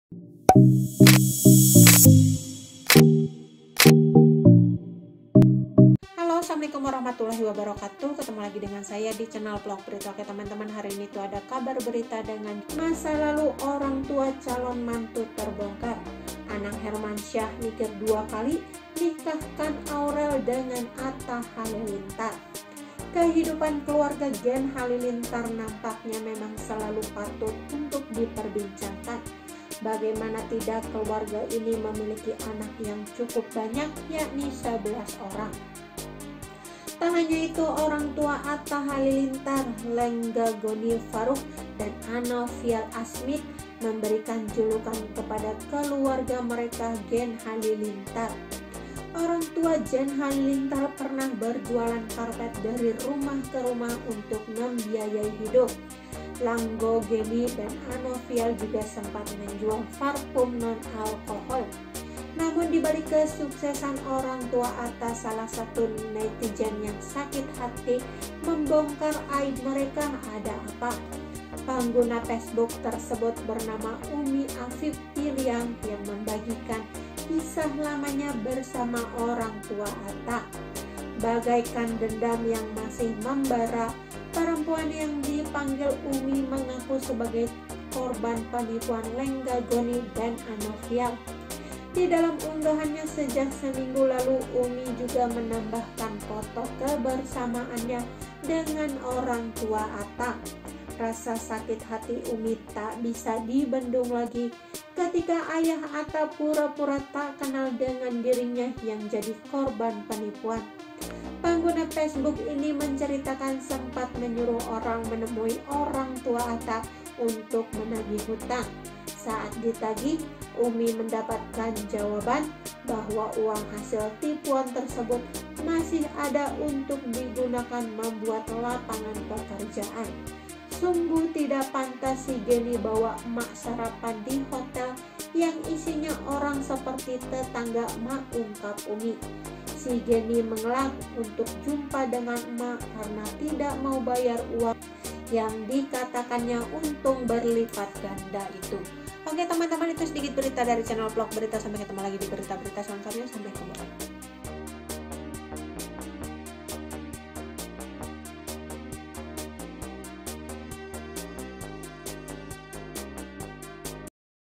Masa Lalu Orang Tua Calon Mantu Terbongkar, Anang Mikir Dua Kali Nikahkan Aurel dengan Atta
Kehidupan keluarga Gen Halilintar nampaknya memang selalu patut untuk diperbincangkan.Bagaimana tidak, keluarga ini memiliki anak yang cukup banyak yakni sebelas orang.

Tak hanya itu, orang tua Atta Halilintar, Lenggogeni Faruk dan Anofial Asmid, memberikan julukan kepada keluarga mereka, Gen Halilintar.

Dikutip dari Nakita, orangtua Gen Halilintar pernah berjualan karpet dari rumah ke rumah untuk membiayai hidup.

Langgogeni dan Anofial juga sempat menjual parfum non alkohol

Namun, di balik kesuksesan orangtua Atta, salah seorang netizen yang sakit hati membongkar aib mereka, ada apa?

Pengguna Facebook tersebut bernama Ummi Afif Piliang yang membagikan kisah lamanya bersama orangtua Atta.

Bagaikan dendam yang masih membara, perempuan yang dipanggil Ummi mengaku sebagai korban penipuan Langgogeni dan Anofial.

Di dalam unggahannya sejak seminggu lalu, Ummi juga menambahkan foto kebersamaanya dengan orangtua Atta.
Rasa sakit hati Ummi tak bisa dibendung lagi ketika ayah Atta pura-pura tak kenal dengan dirinya yang jadi korban penipuan.

Pengguna Facebook ini menceritakan sempat menyuruh orang menemui orangtua Atta untuk menagih hutang.

Saat ditagih, Ummi mendapatkan jawaban bahwa uang hasil tipuan tersebut masih ada untuk digunakan membuat lapangan pekerjaan.

Sungguh tidak pantas, si Geni bawa emak sarapan di Hotel yg isinya orang orang seperti tetangga emak," ungkap Ummi.

Si Geni mengelak utk jumpa dengan emak. Krn tidak mau bayar uang yg dikatakannya untung berlipat ganda itu.